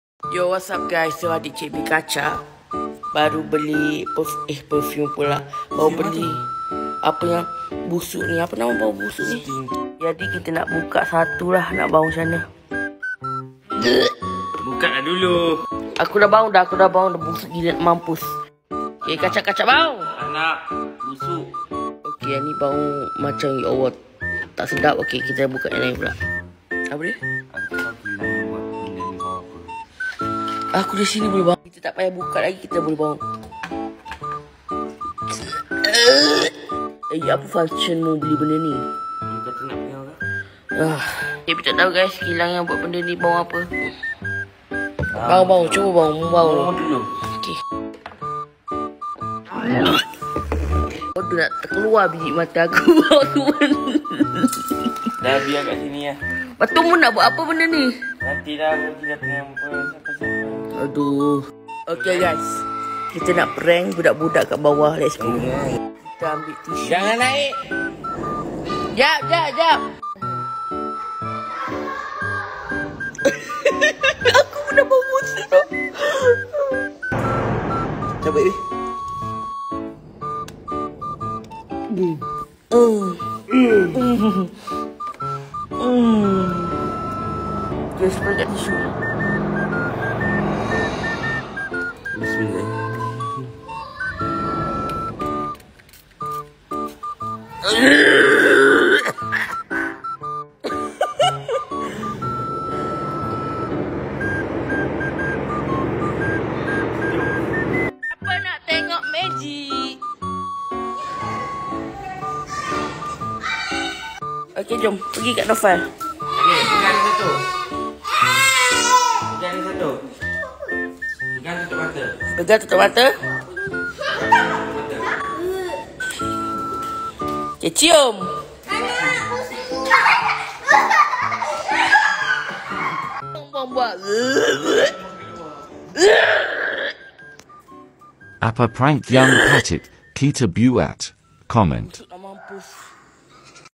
Drama Part 686 | BILA PERFUME BAU NERAKA : SEKALI INI REAKSI CIKB - KACAK PULA BIKIN INI
Bila anak jutawan kosmetik kontroversi, Dato Seti Vida, CikB mengidu perfume tetapi bau busuk macam neraka... lalu kacak bikin ini...

info kredit
titok cikb havoc